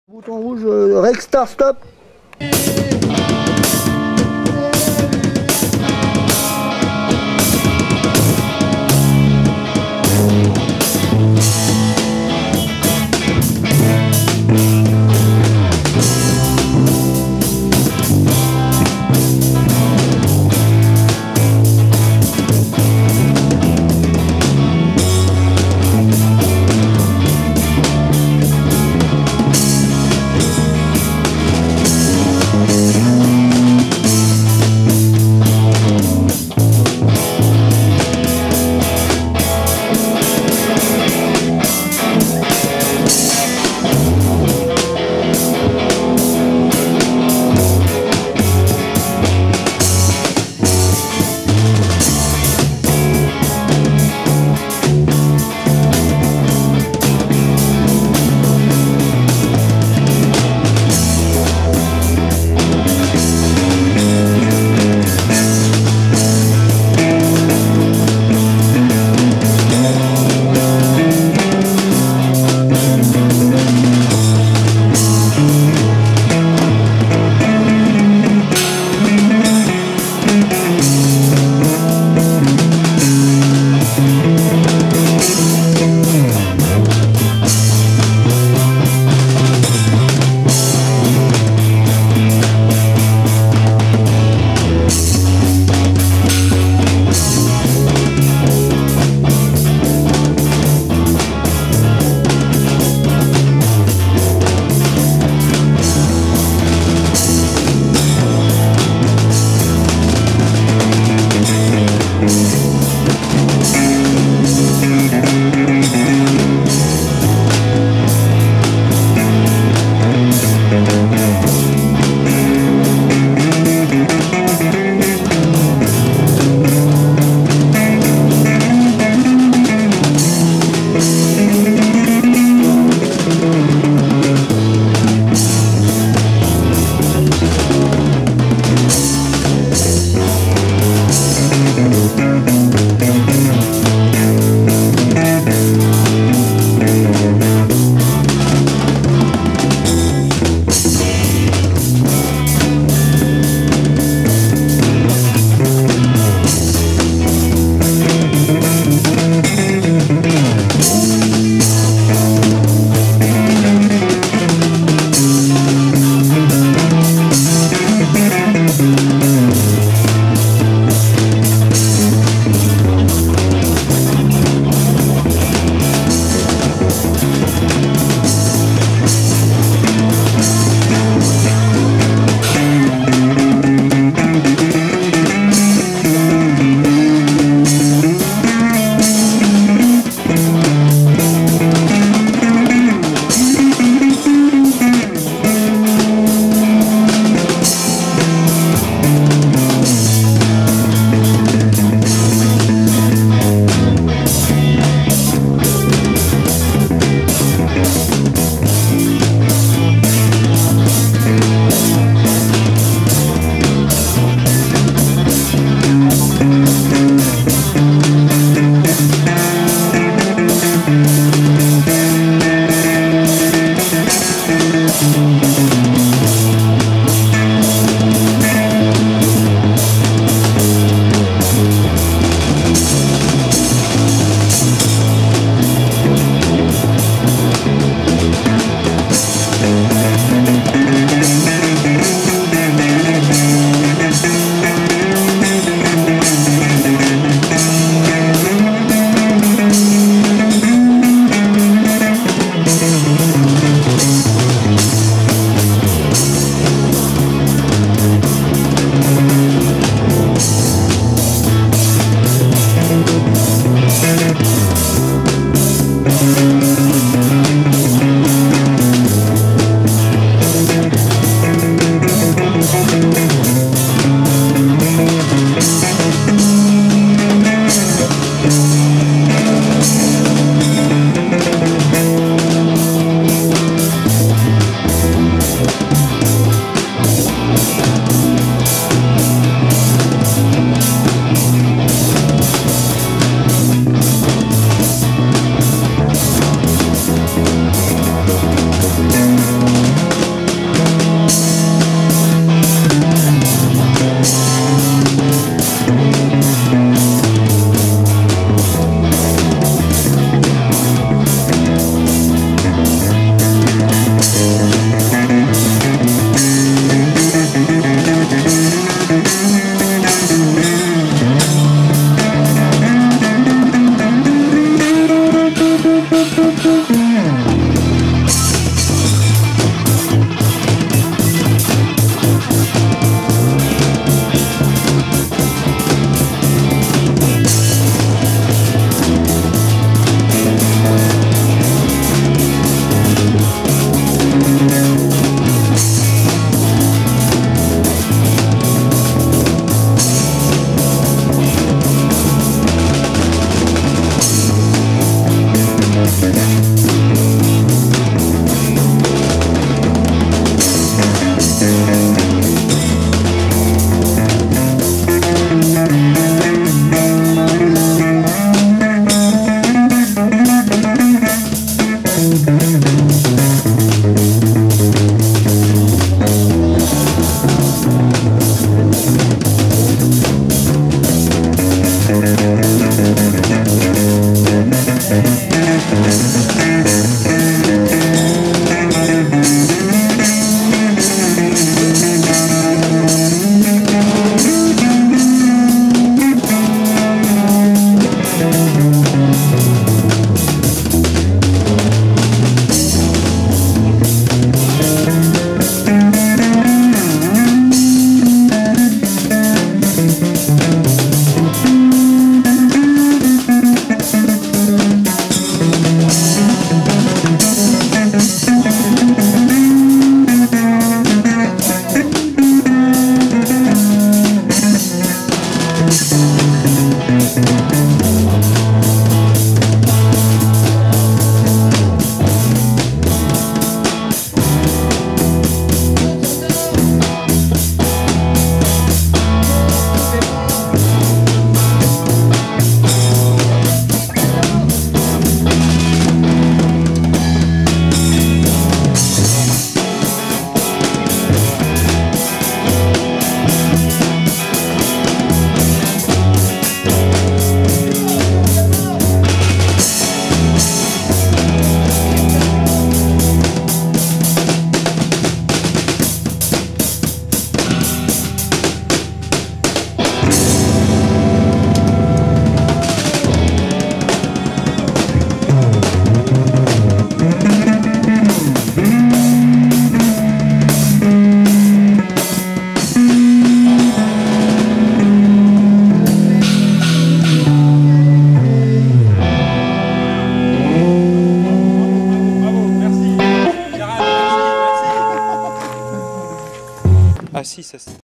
Gérald 03
Gérald  batteur furieux de Die  levitablocs le 21 Juin 2018 à Die boulettes de rocks chutes de pierres rochers-qui-tombent écrasement-rocks pierres-éclatées roulage-caillouteux concassage-ruth-nique-en-poly-coulures de notes Guitare Bernard Batterie Jeanphi et Basse nnartin autoprod